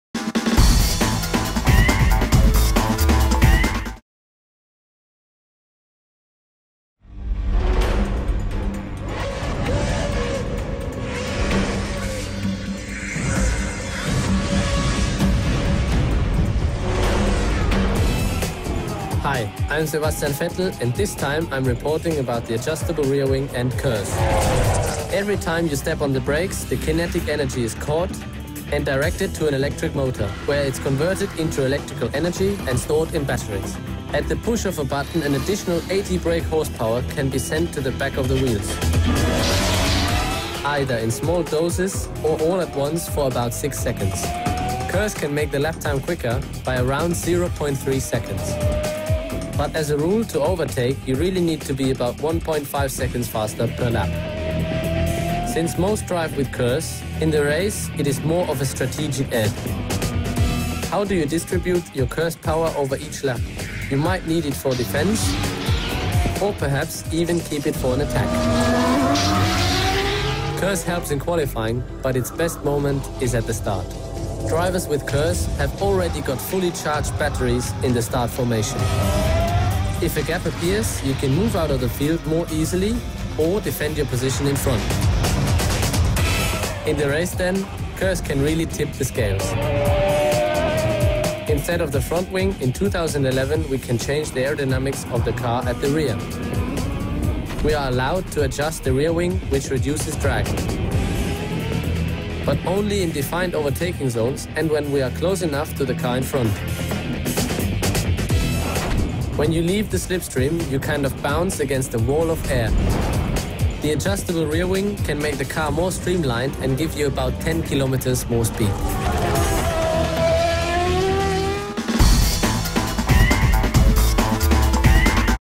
Sebastian Vettel Explains KERS and DRS
World Formula One Champion, Sebastian Vettel explains the Kinetic Energy Recovery System and DRS (Drag Reduction System).